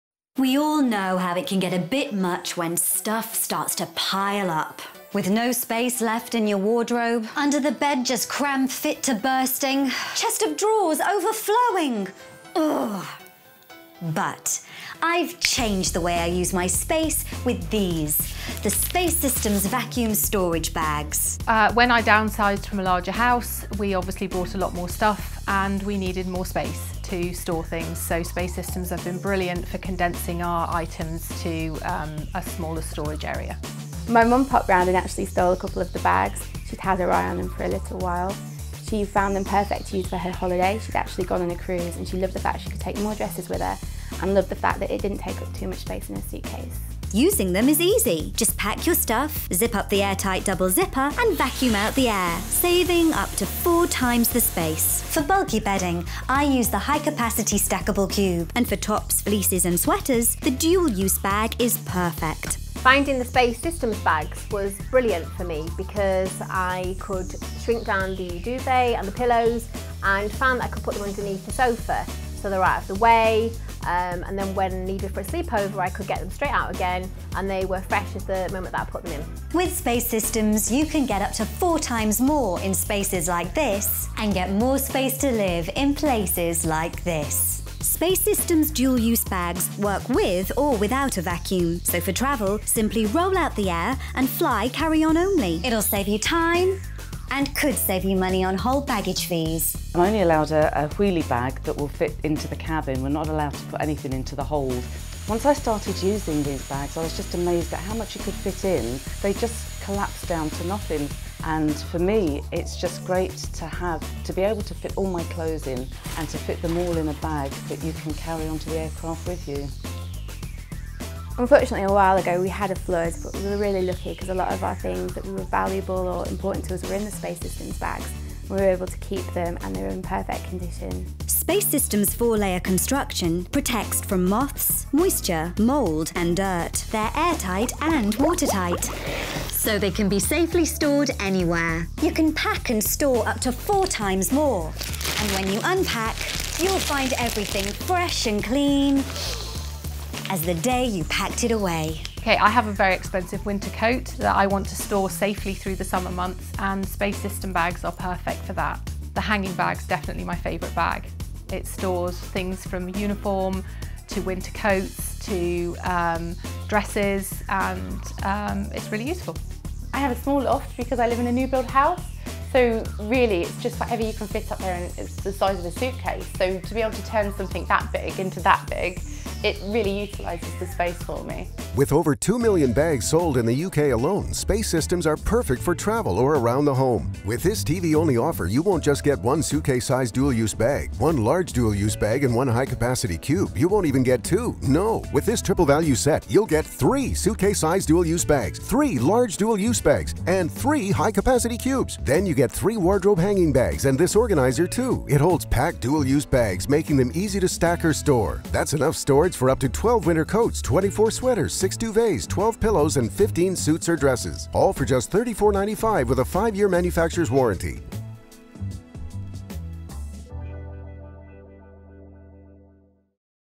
Space Systems 2016 ONLINE 34 95
Online spot for Space Systems vacuum storage bags. Used on the buyspacesystems.co.uk website to support the broadcast commercial.